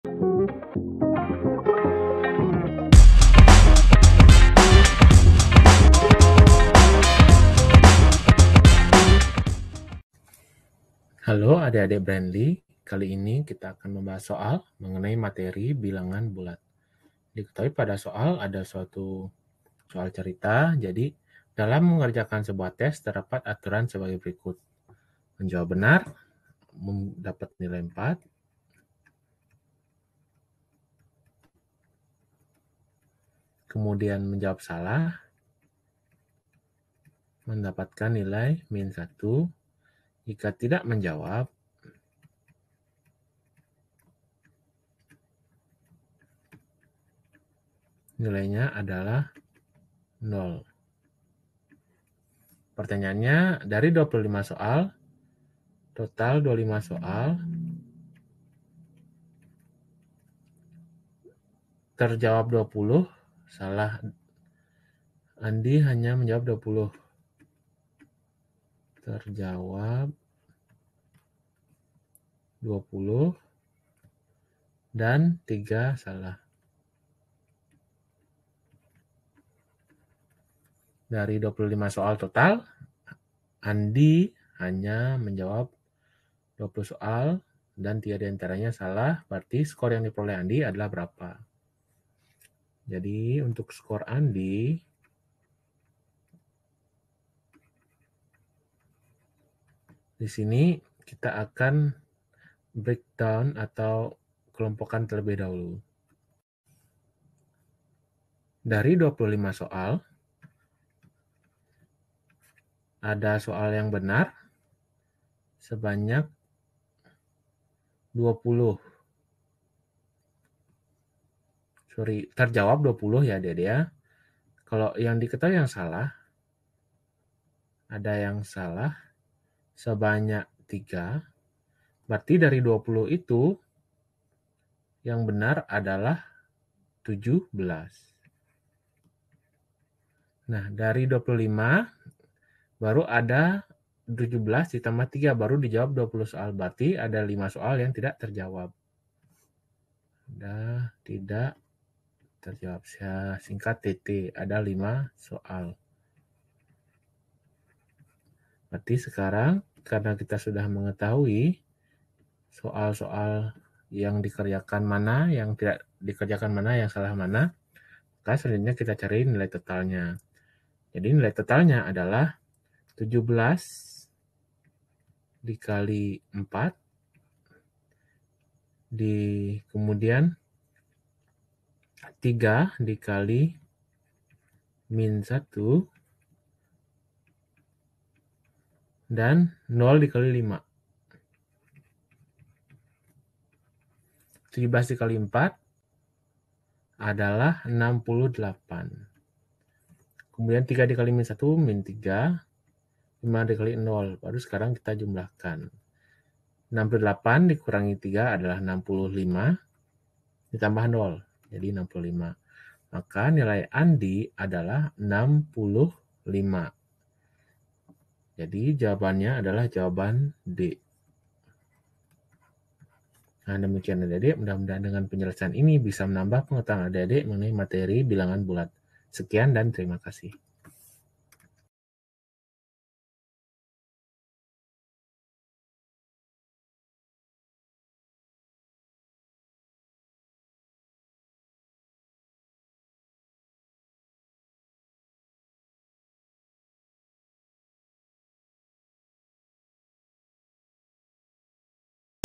Dalam mengerjakan sebuah tes terdapat atur... | Mandiri Matemat | Kelas 7 | 1 | Matematika
✅ Dalam video ini kita akan membahas: Dalam mengerjakan sebuah tes terdapat aturan sebagai berikut:
Menjawab benar = +4
Menjawab salah = -1
tidak menjawab = 0
Dari 25 soal, Andi hanya menjawab 20 soal dan 3 di antaranya salah. Skor yang diperoleh Andi adalah . . .
A. 77   C. 73
B. 75   D. 65
Tema:
Kelas: 7
Mata Pelajaran: Matematika
Bab: 1
Buku: Mandiri Matematika Untuk SMP/MTs kelas VII / 1

👉 Buku yang telah tersedia:
{PLAYLIST_LINK}
Di mana kamu dapat menemukan kami?